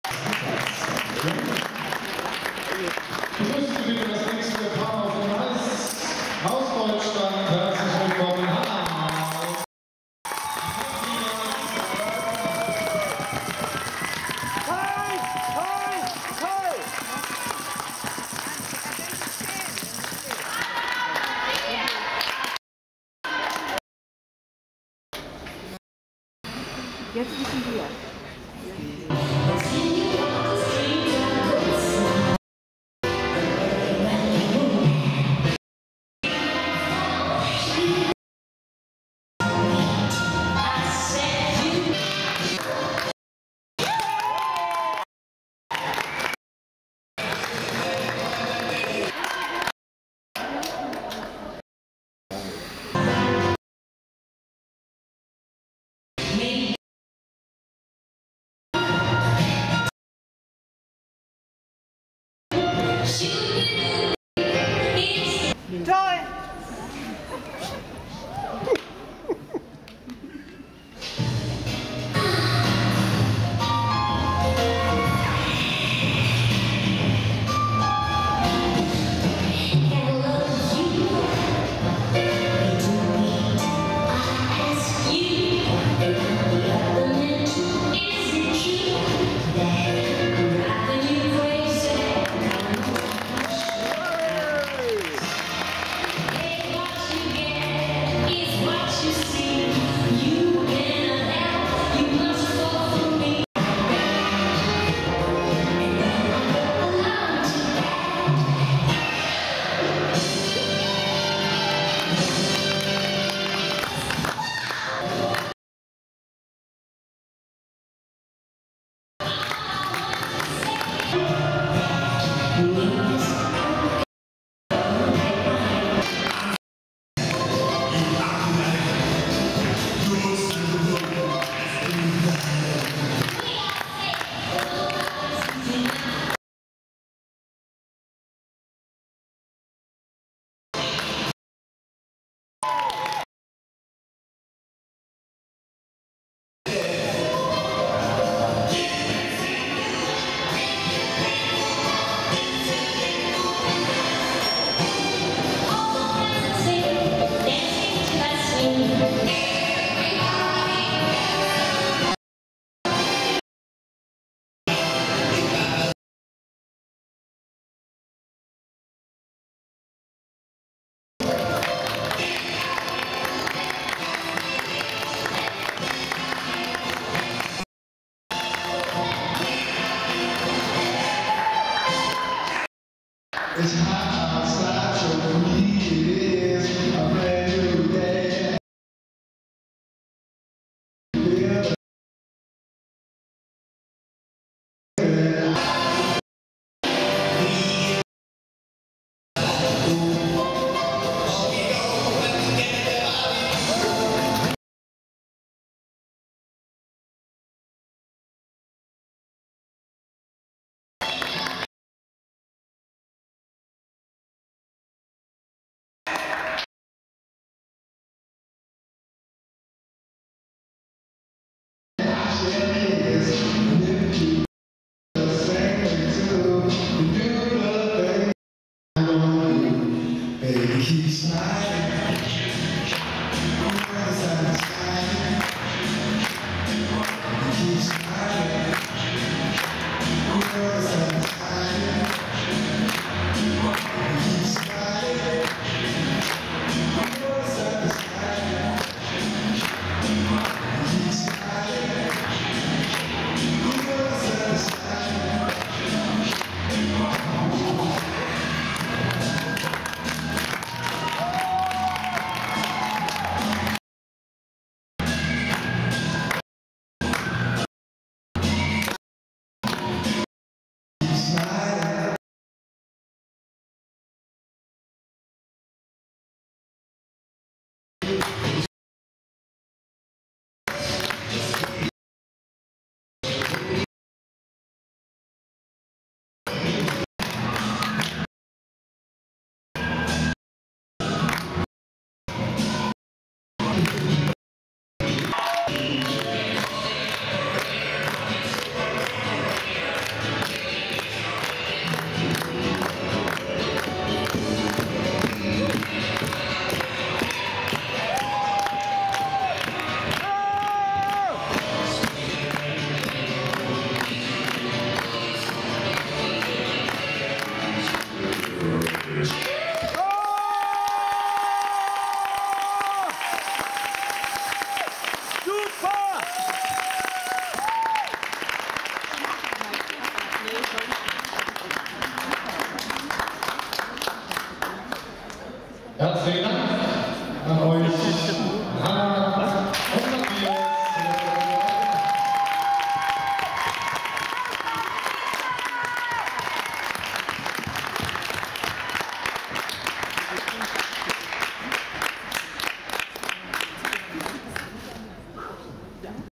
Hannah u. Matthias Rauthmann NRW Trophy 2010 Eistanzkür (Freedance) Junior
Eistanzkür NRW Trophy 2010
Hannah und Matthias Rauthmann
Dortmund